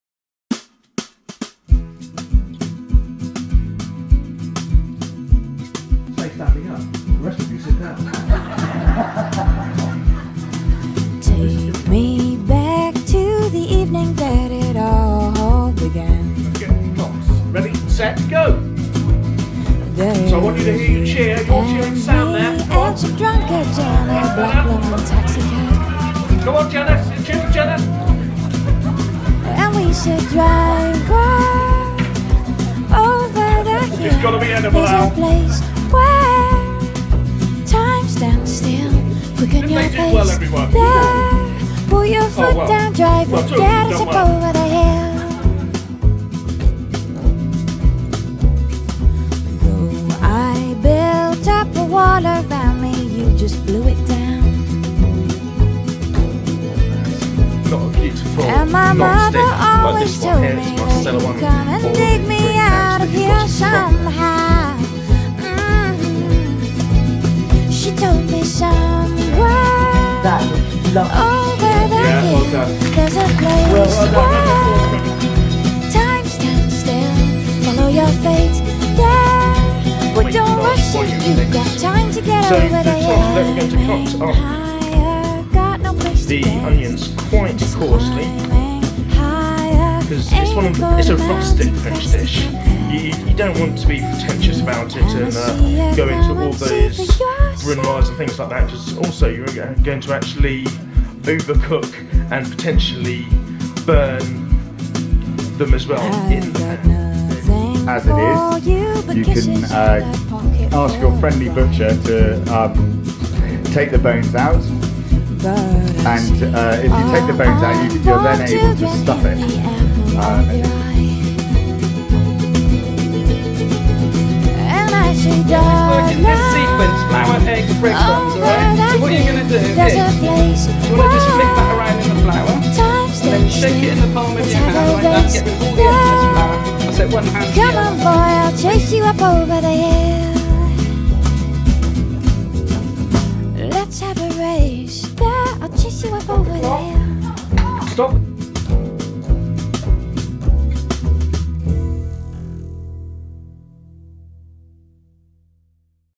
Weekend Spectacular Saturday 2015
Highlights from day one of our Weekend Spectacular & Mouse Trap Theatre in association with the Sturminster Newton Cheese Festival